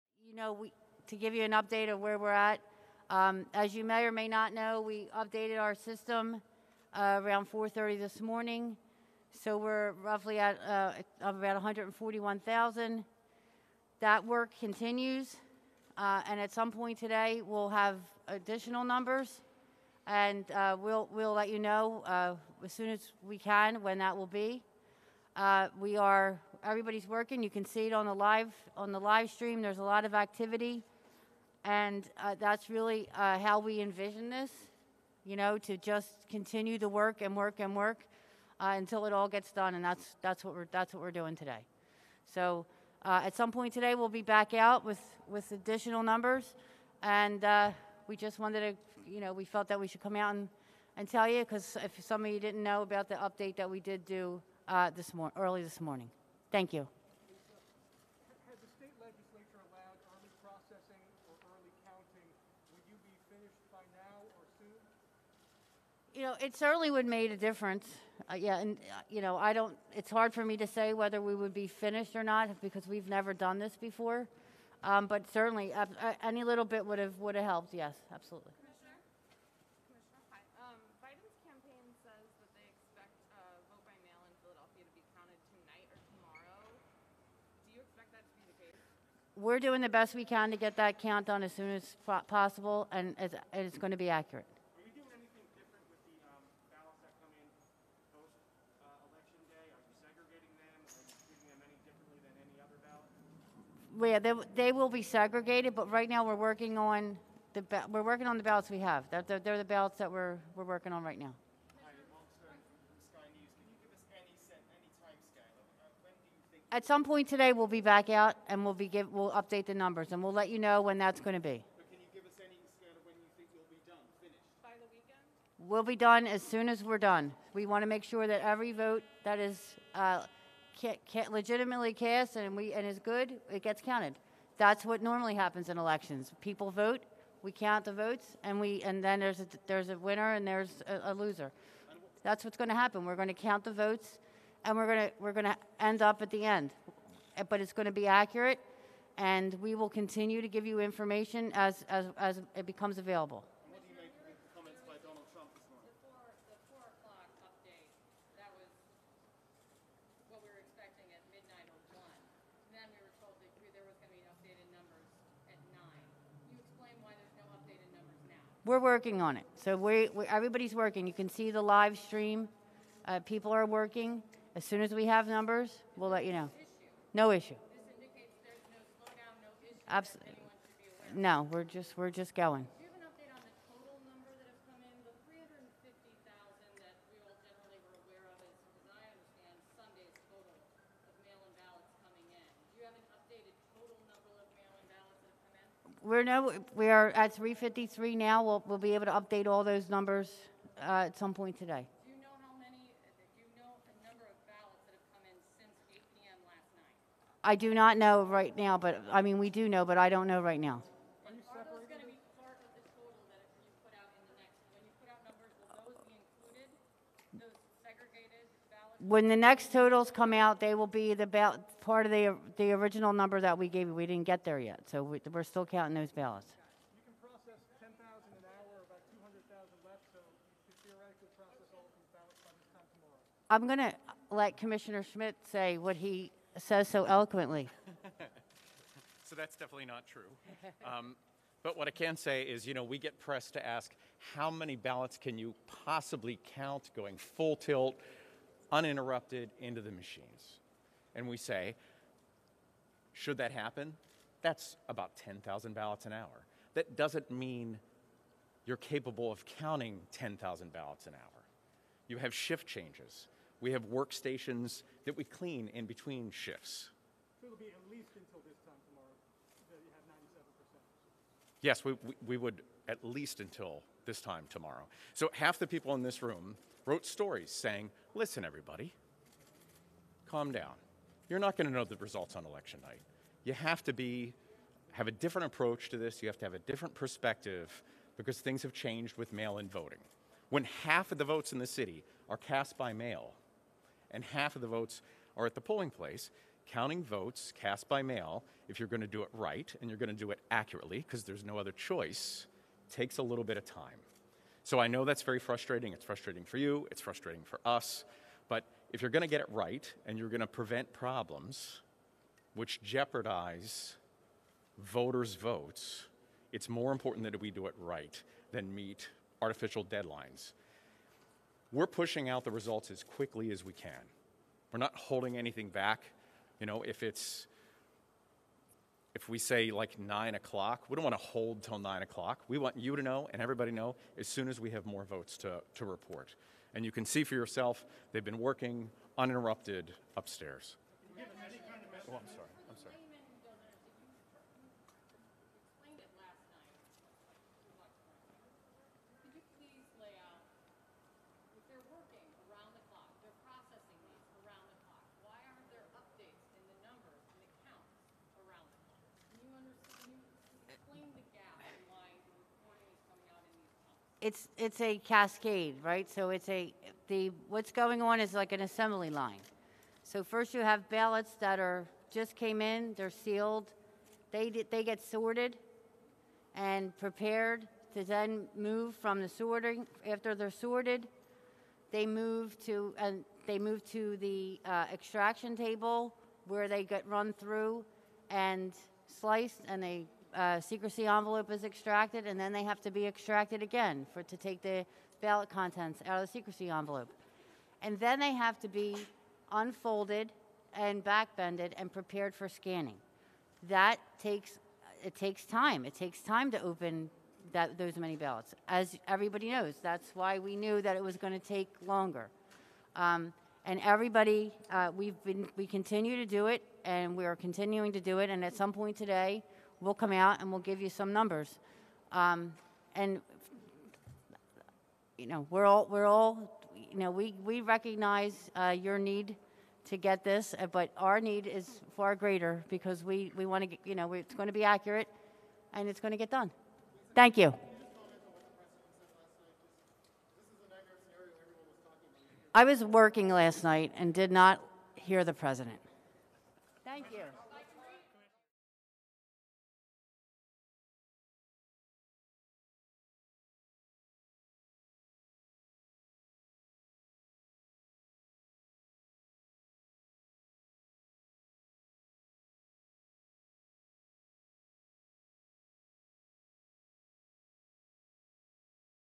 Philadelphia election officials provide updates on ballot counting
Philadelphia election officials speak to press and provide updates on ballot counting.
US Election Result , US Election Voting , US Election Result Update